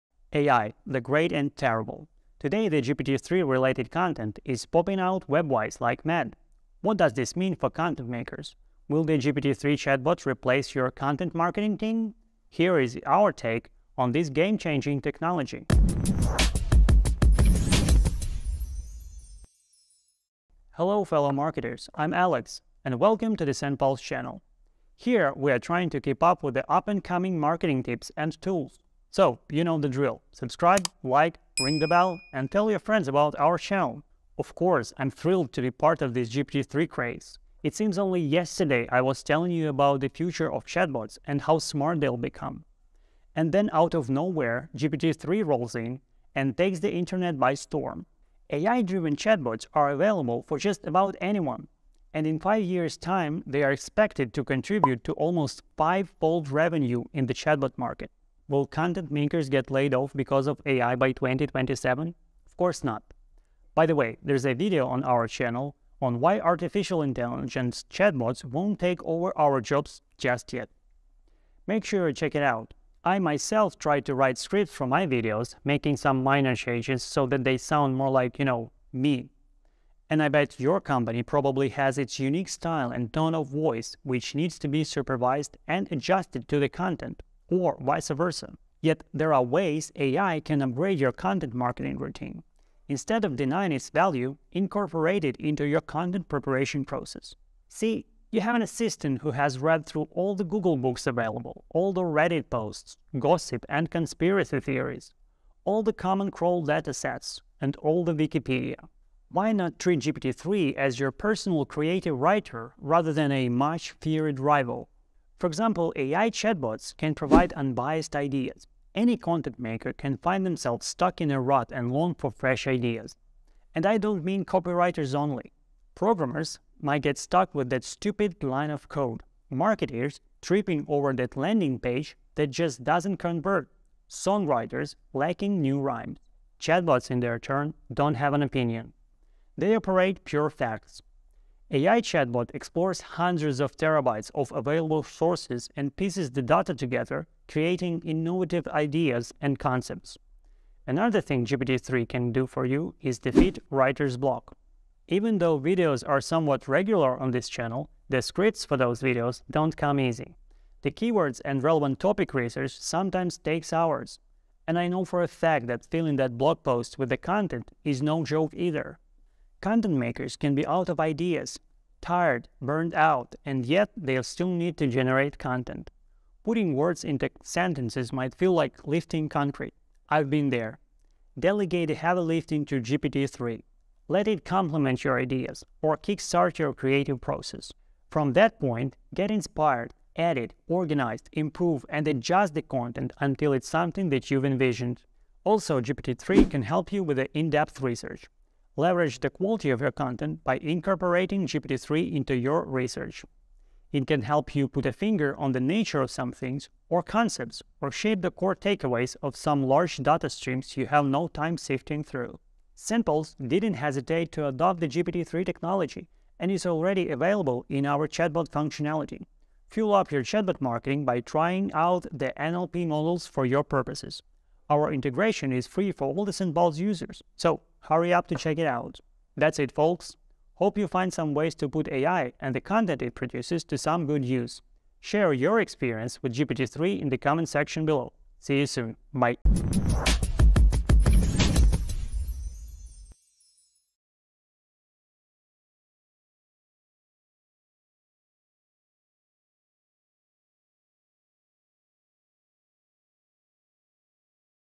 Can AI Create Better Quality Content in 2023
In this video, Alex explores the exciting topic of whether AI can create better quality content in 2023. With the rapid advancement of technology, many businesses are turning to marketing automation tools like SendPulse to streamline their content marketing efforts. But what if AI could take it one step further and create content on its own?
Alex delves into the capabilities of AI to generate written and visual content, discussing the potential benefits and drawbacks of using content AI in 2023. As a language model trained by OpenAI, ChatGPT also makes an appearance to provide insights on the topic. Don't miss this thought-provoking discussion on the future of content creation!